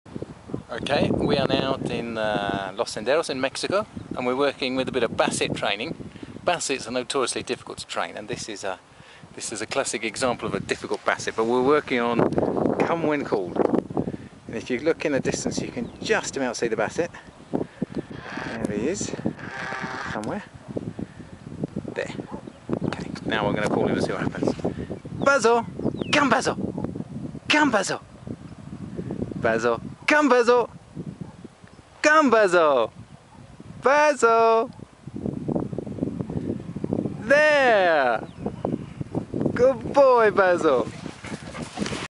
Basset hound training
Basil the Basset hound shows off his skills in hesitant obedience